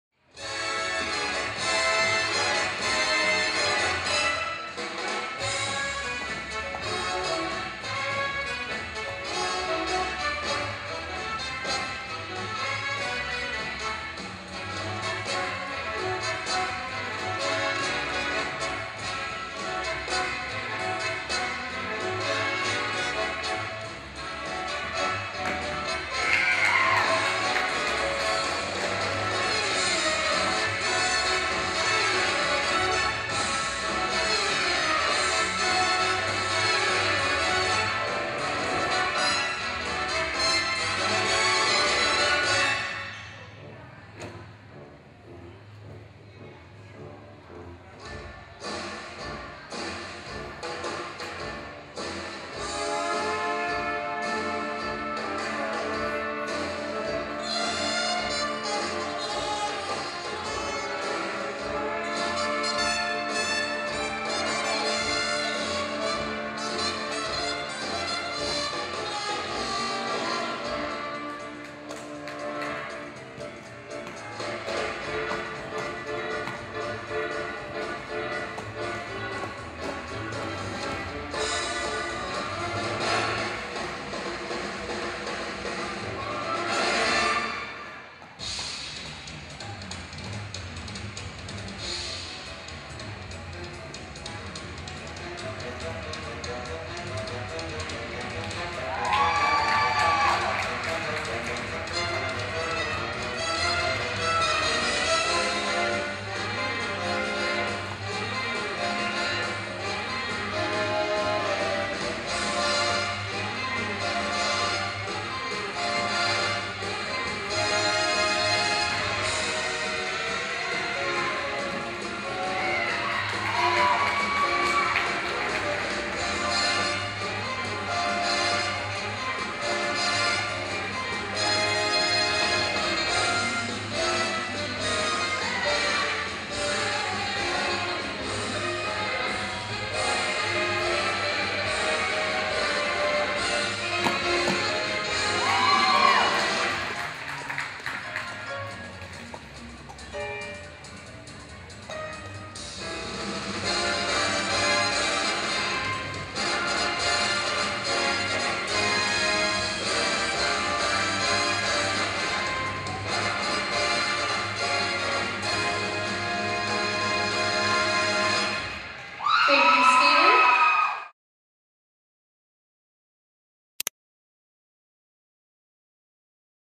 Skating Competition - Lynwood 2018 : Samantha Krusza
Part of Highlights from IIWAL Skating competition 2018 - Lynwood
The competition took place at the Lynwood Skating rink and featured several Lansing residents.